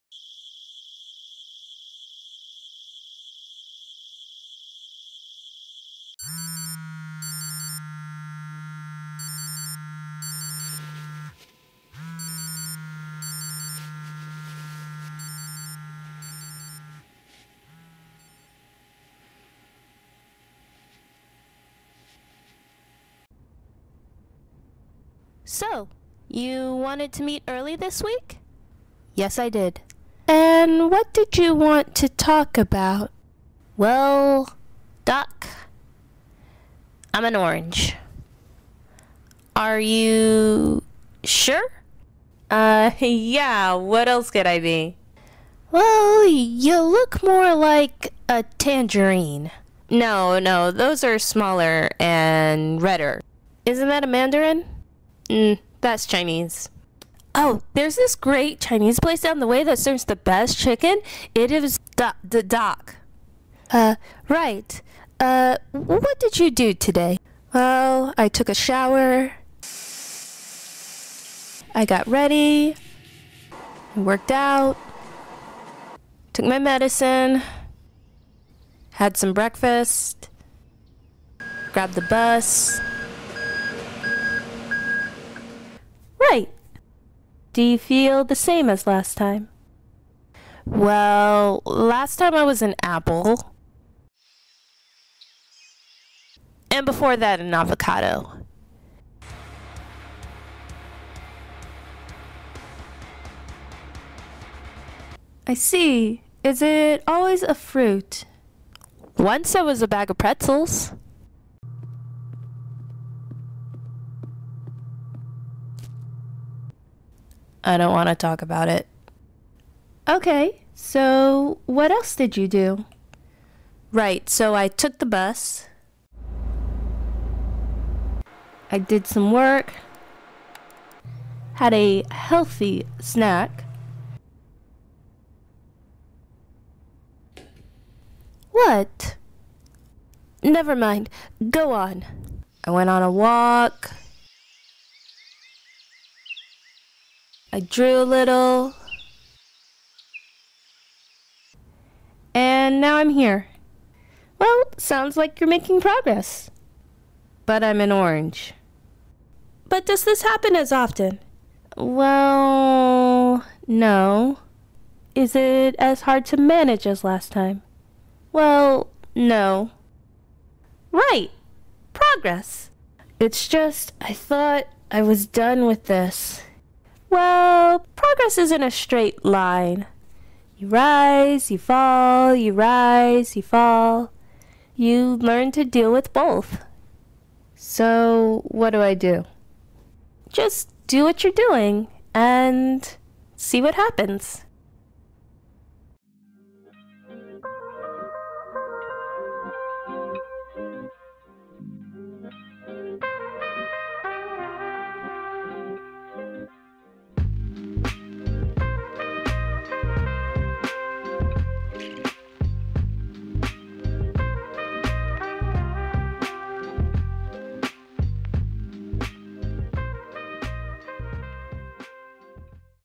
It’s a Spiral or I Woke Up As An Orange
An important thing to remember during recovery.

Created by Marie Jarrell at Clemson University in 2019 as part of Campus MovieFest, the world’s largest student film festival.

Cast & Crew:
Marie Jarrell – Captain